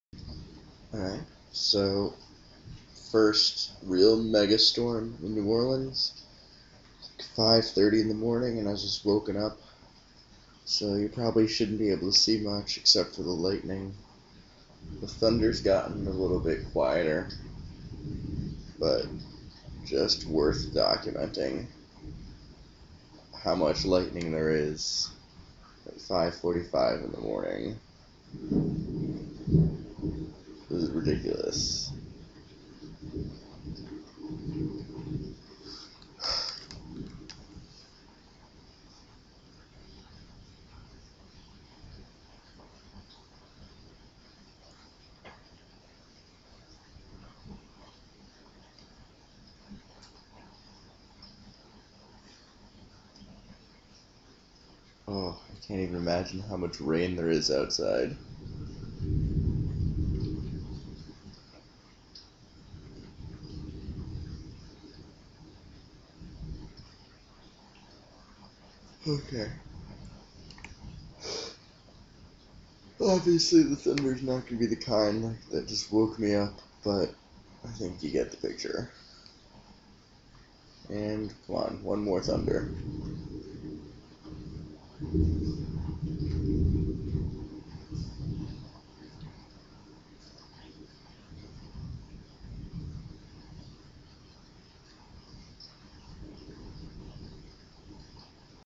Lightning in New Orleans
it rained really hard really early in the morning.